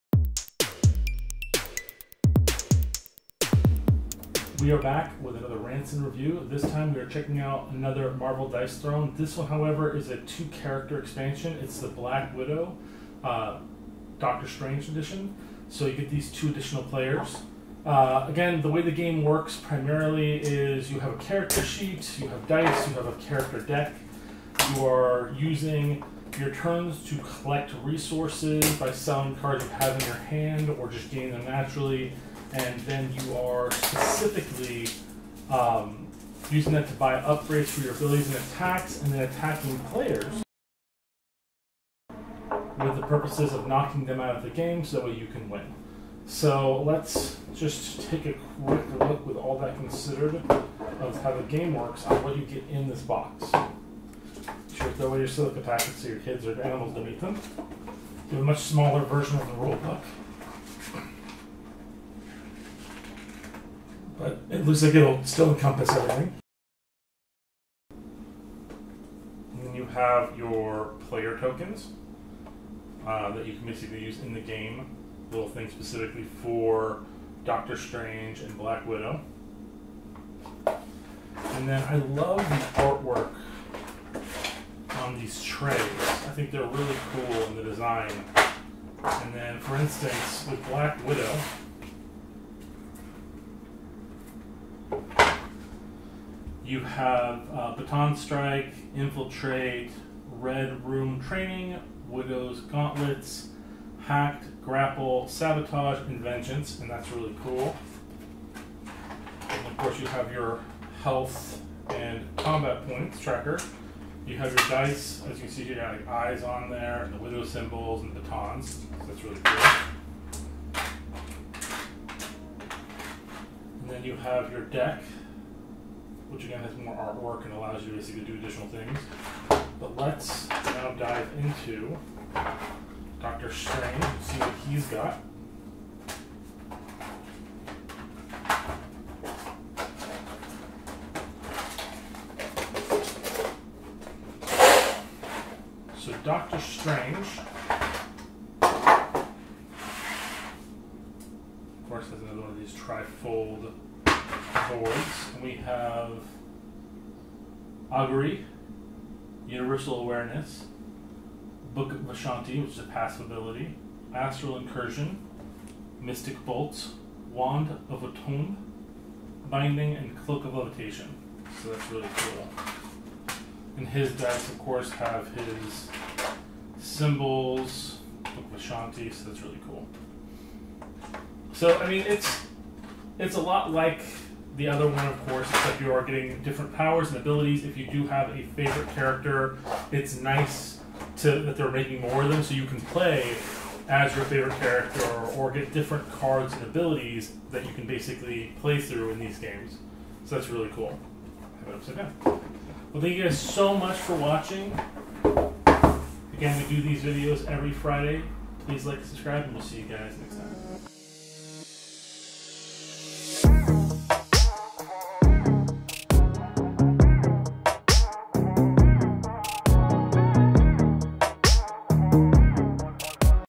Rants And Reviews: Marvel Dice Throne - Black Widow / Doctor Strange Expansion
Keeping our promise, it's Friday, and it's time for another Rant and Review of some of our favorite board games!

This week we are checking out an expansion for Marvel Dice Throne by The Op Games. This expansion is the two character Black Widow / Doctor Strange Expansion. See our prior review of the base game here:

This is a part deck-builder, part dice-roller game which allows you to take over the abilities of a super hero in the world of marvel. The winner of the game is the one who knocks out the other players. This game is played in teams, or played like a king of the mountain.